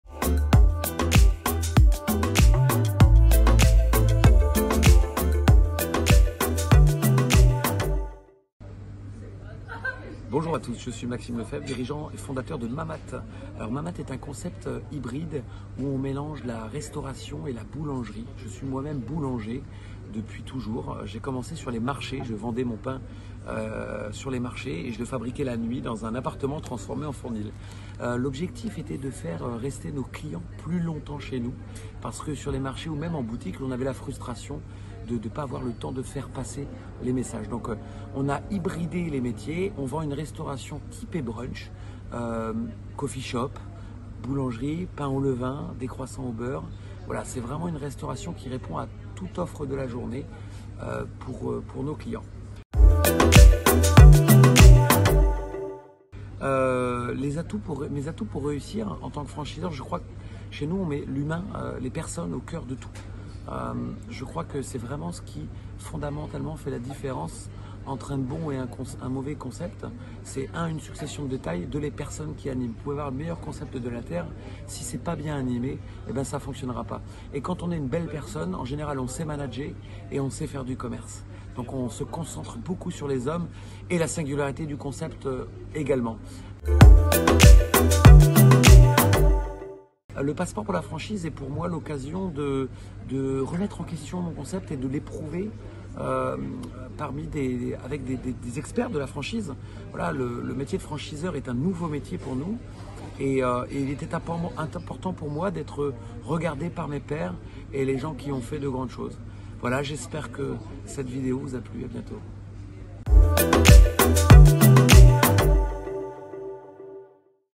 Mamatte : finaliste du Passeport pour la franchise 2024
Le concept de Mamatte, à mi-chemin entre la boulangerie et le restaurant, a séduit les partenaires du Passeport pour la franchise 2024